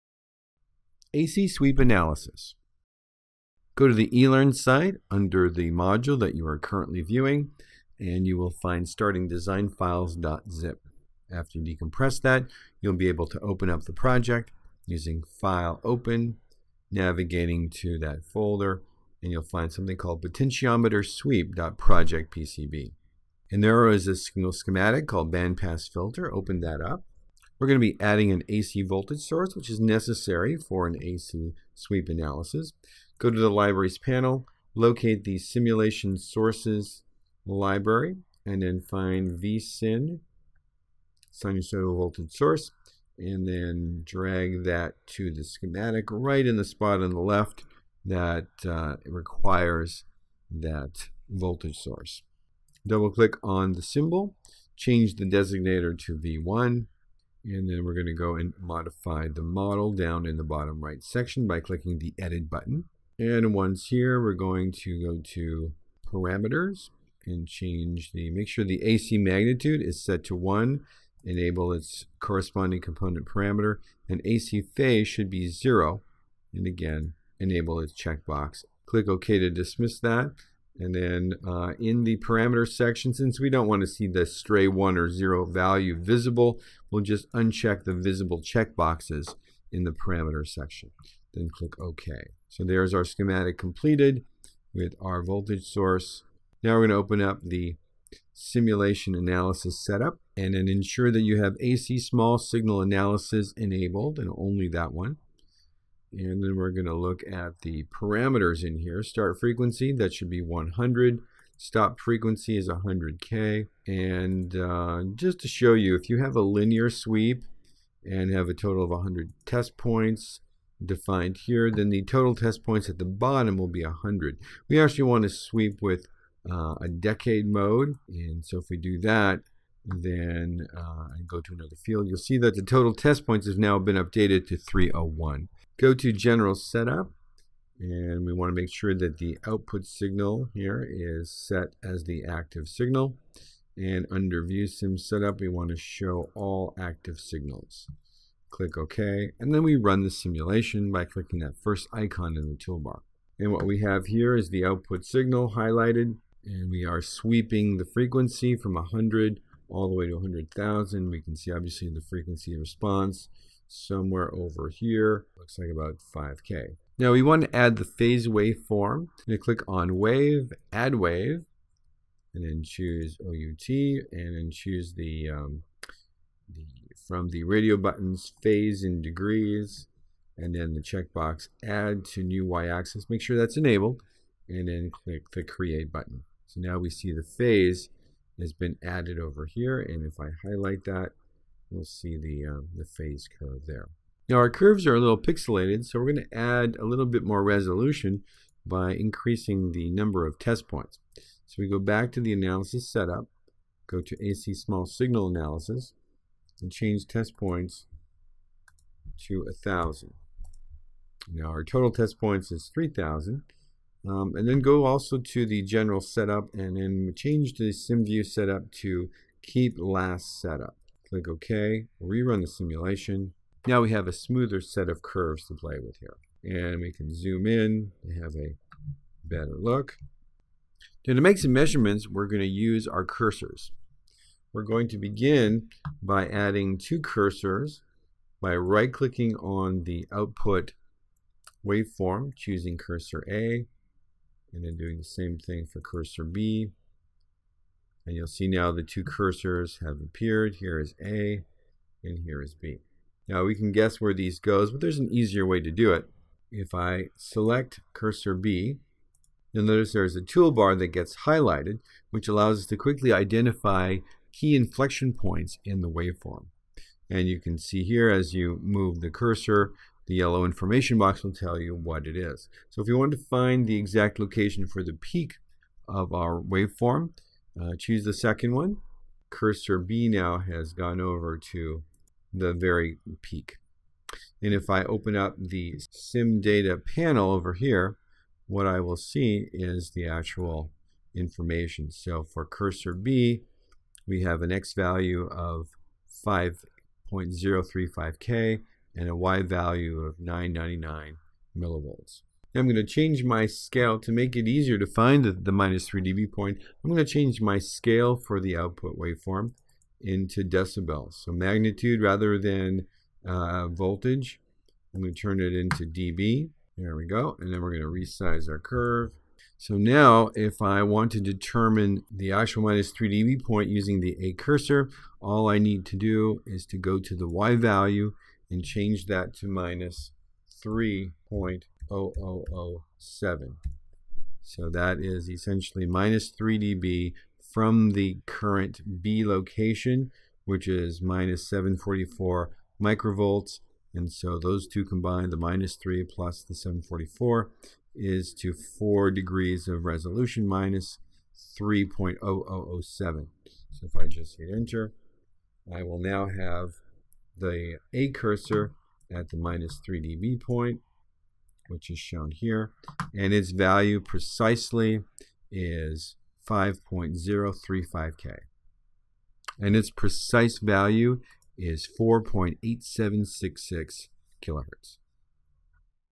How to perform SPICE AC Sweep analysis - Module 6b
In this Altium Designer (2015) SPICE simulation training module we will cover AC Sweep analysis - one of the most frequently used analysis types, which results in the frequency response plots of an analog circuit.